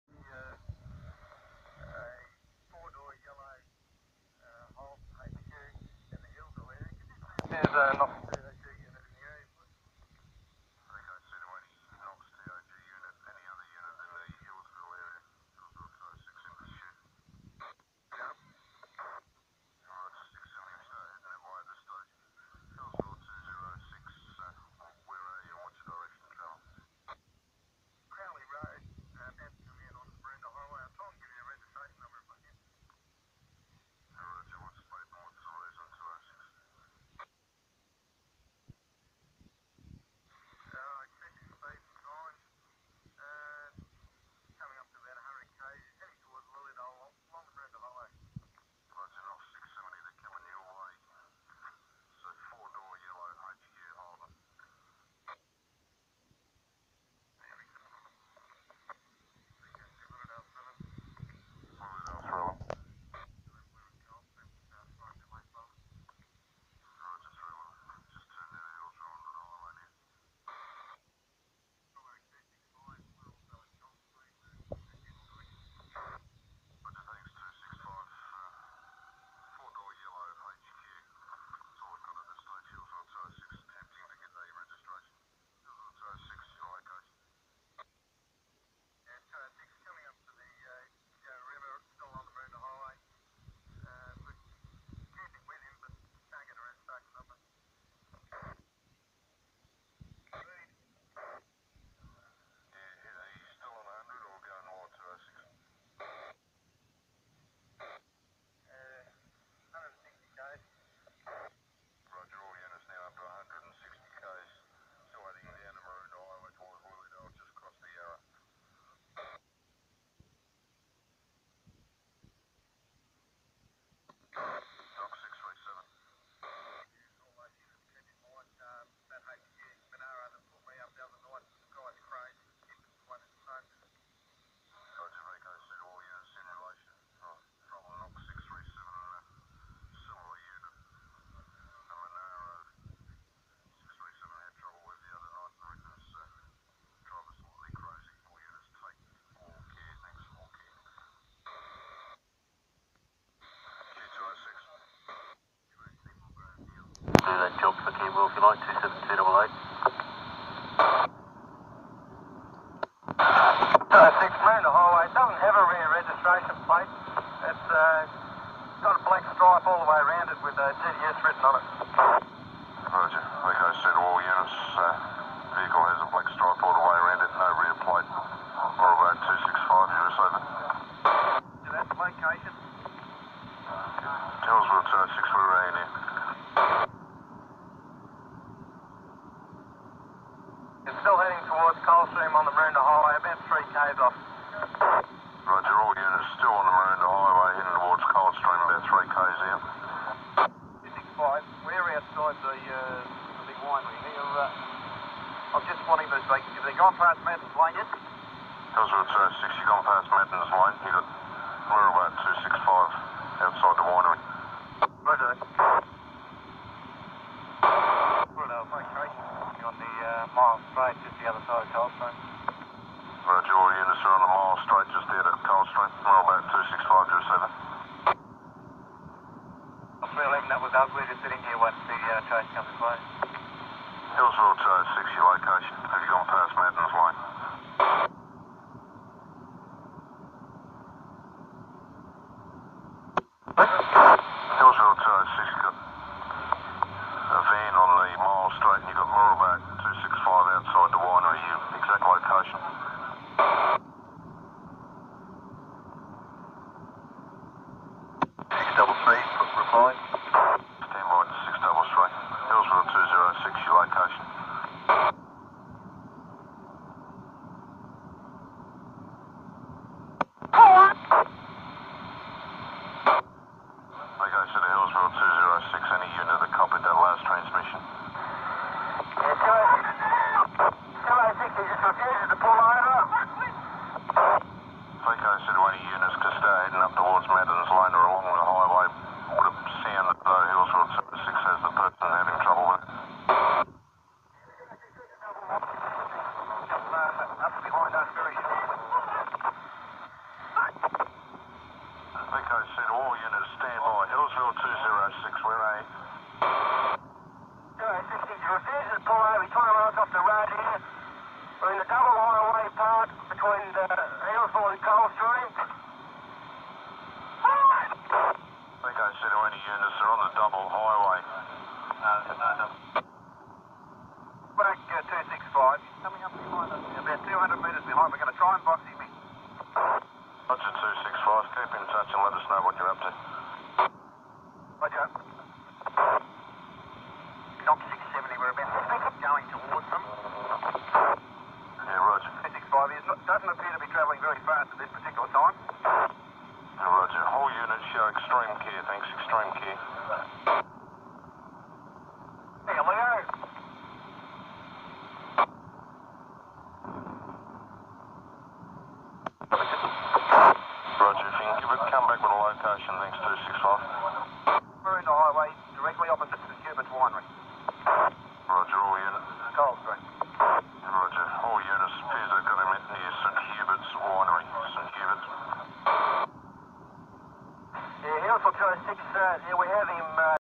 Police chase audio ! Melbourne , Vic , Australia , 1990 !
Poor quality sound then becomes better later on ! Recorded on a crap phone off a 30 year old cassette tape ! Police want to pull up an old Holden monaro ! Now a classic worth heaps ! He won't stop ! You can hear cops yelling and swearing at him ! I might try and reupload this with better sound quality  see what I can do ? But from 2-40 its a lot louder !